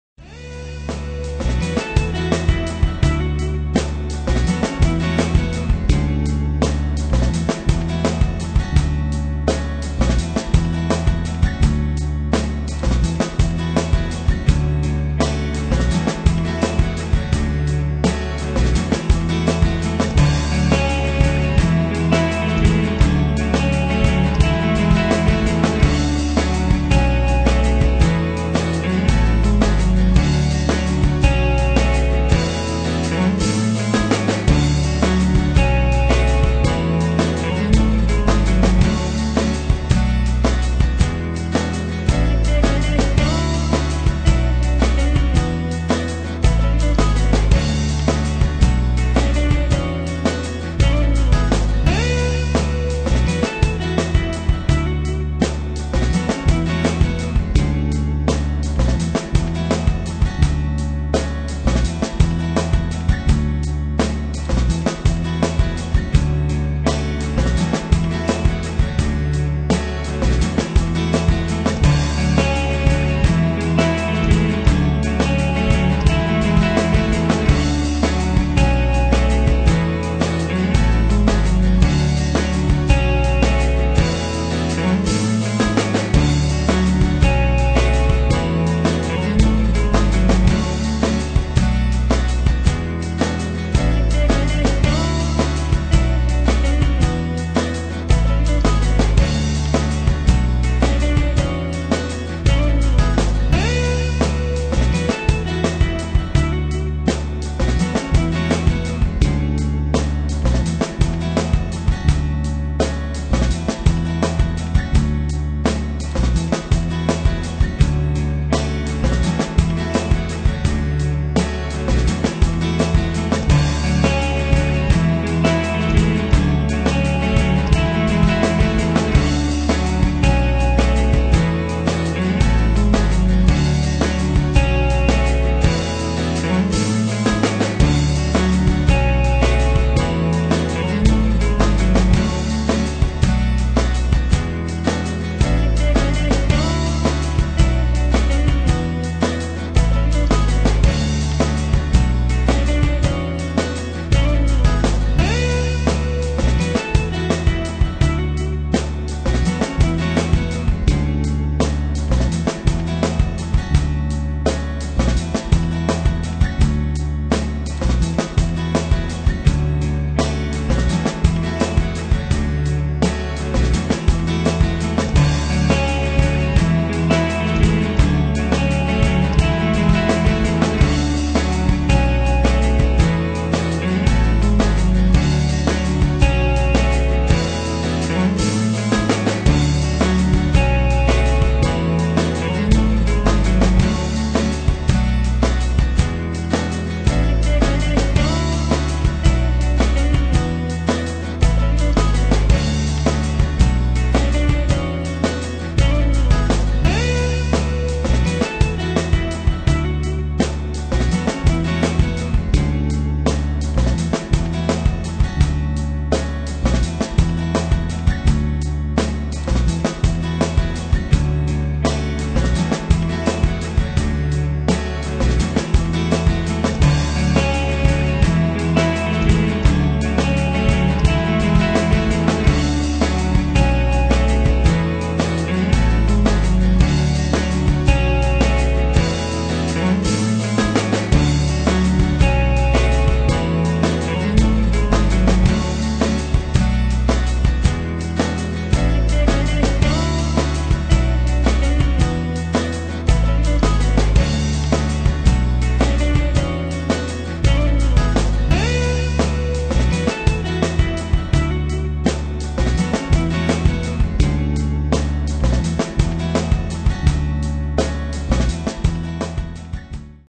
Chinook Fun at the Greenwich Kennel Club Show
Photos from the Chinook Club of America's Supported Entry at the Norwalk, CT Show on June 9, 2012